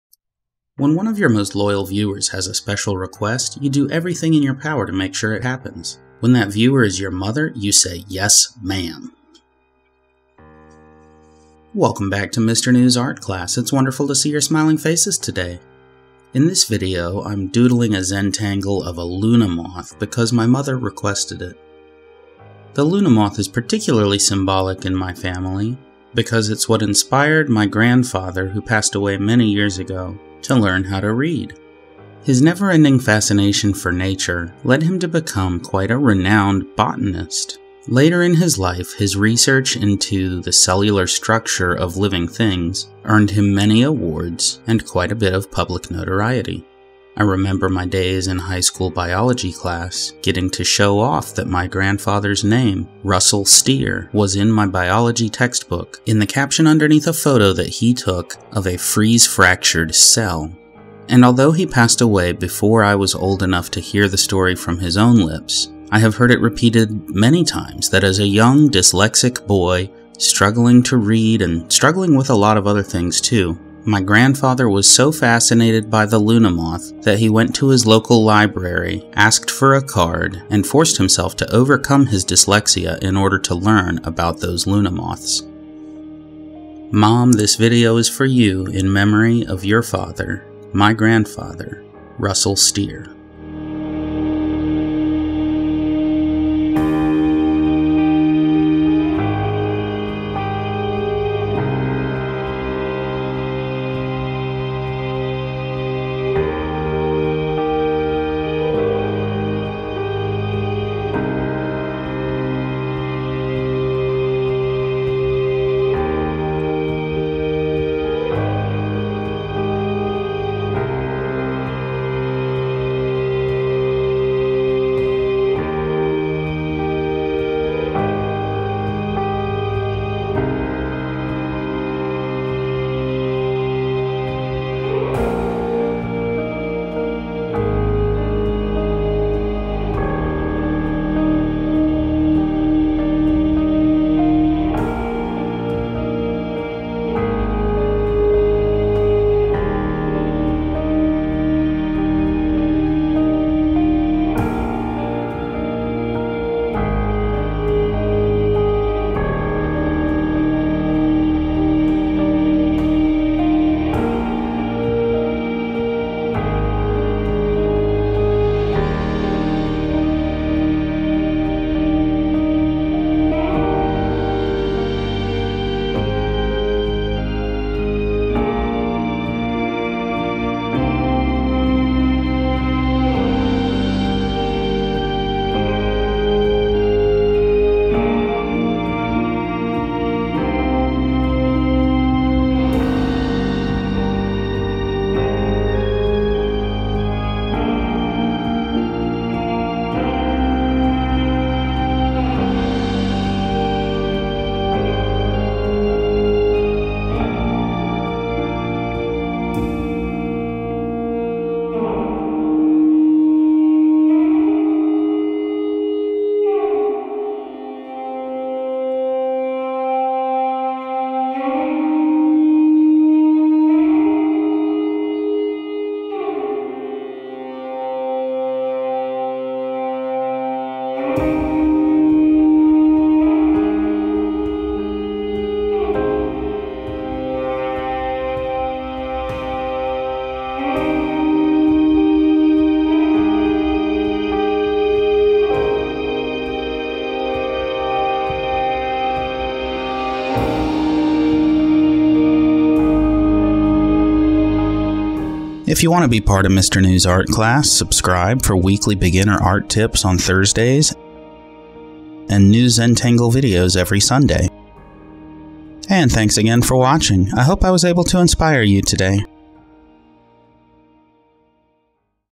ZenDoodling a Luna Moth - For Mom, A Tribute to Gramps - Weekly Zentangle Tutorial Vol. 13
Learn something new with Mr. New! In this video, I doodle a Luna Moth for my mother, in memory of her father (my grandfather). I began this drawing by looking up references of Luna Moths, and using a pencil to create an outline sketch of the insect. Then, I used a large-size pen to doodle some textures into the darkest areas of the moth. After that, I went over the veins inside the wings with a simple path-following pattern of squares. Finally, I filled the remaining spaces with light-value patterns using my smallest pen. I also applied some shading to a few areas to make them stand out.

The materials used are:

Ticonderoga Pencils
Prismacolor Fine Liners
X Press It Blending Card (25 pk)
X Press It Blending Card (125 pk)